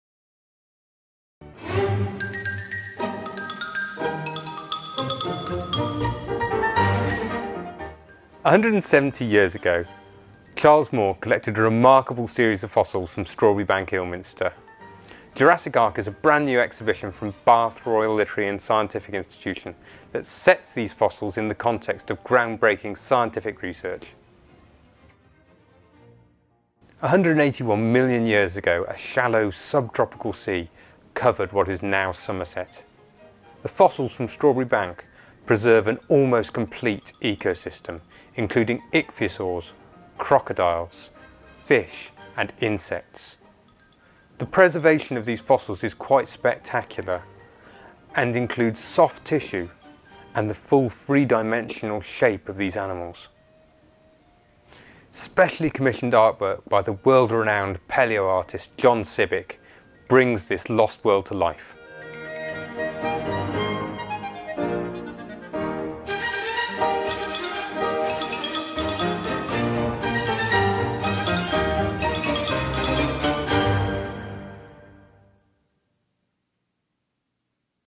Jurassic Ark Trailer
The BRLSI celebrates its unique collection of fossils from the Lower Jurassic of Strawberry Bank, Ilminster, and the work of the JESBI project with an Exhibition from 24th May -- 30th September 2014.

Beneath the Somerset town of Ilminster lies a beautifully preserved Jurassic ecosystem, 181 million years old, from which Victorian geologist, Charles Moore, collected hundreds of fossils. Now this hidden treasure trove has been brought to life in a new exhibition, showing how these creatures lived and interacted, how they died and were preserved, and what they can tell us of the history of life.

Specially commissioned illustrations by John Sibbick, one of the world's foremost palaeontological illustrators, recreate the landscape of the Jurassic period in which the extinct marine reptiles, fish, crustaceans, squid-like cephalopods, and insects flourished.